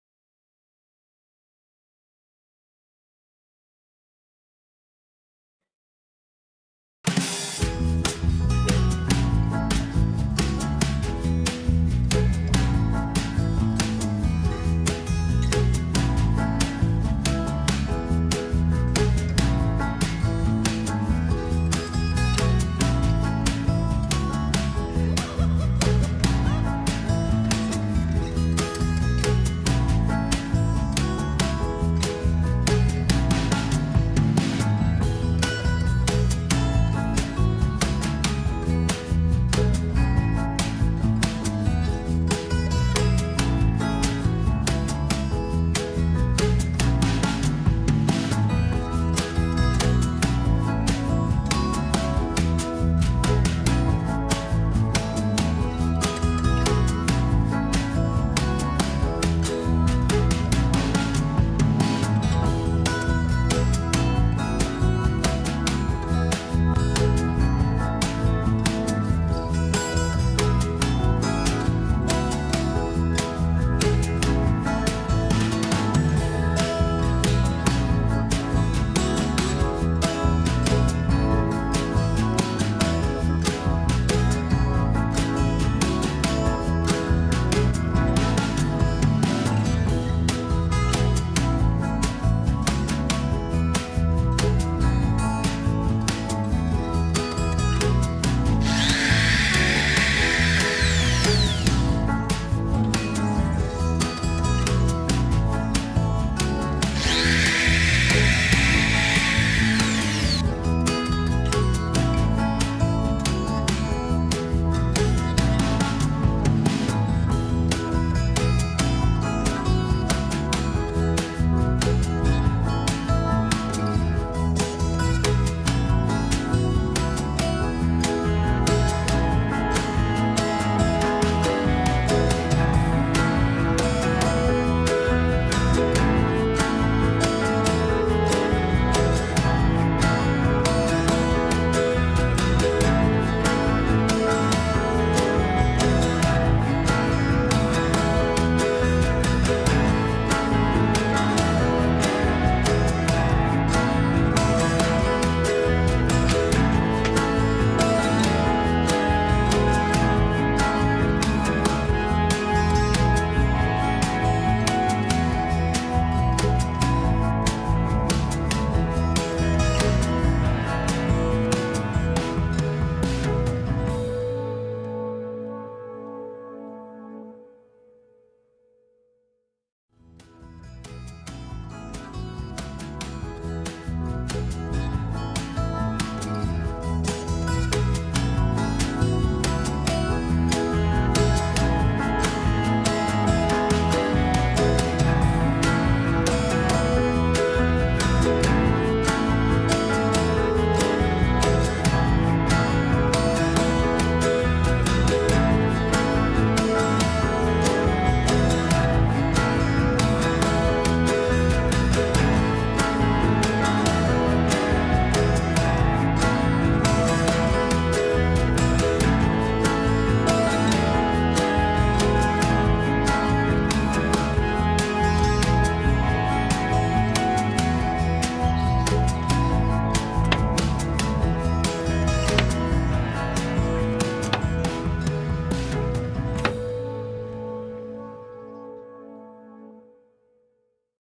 Diy popsicle stick bow || ice cream stick art and craft
Diy popsicle stick bow - today i will show you how to make a mini bow using ice cream sticks. Best out of popsicle stick craft ideas